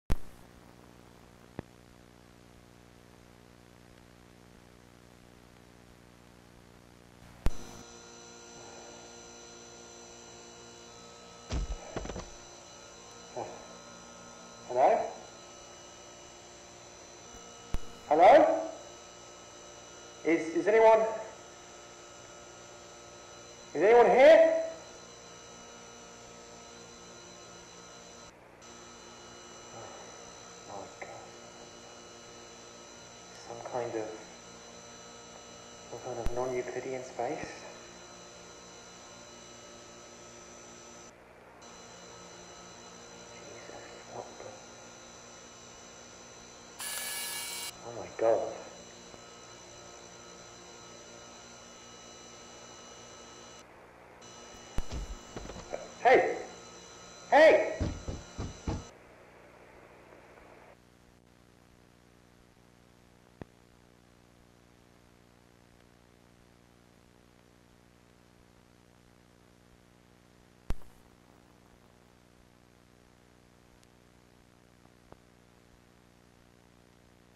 SCP Foundation: The Backrooms
The Backrooms is NOT an SCP, but the universe of the Backrooms and the SCP Foundation have such similar themes that I think they work really well together in a crossover! This video is my take on that crossover, obviously inspired by the amazing @kanepixels  and their Backrooms series, and I hope you like it.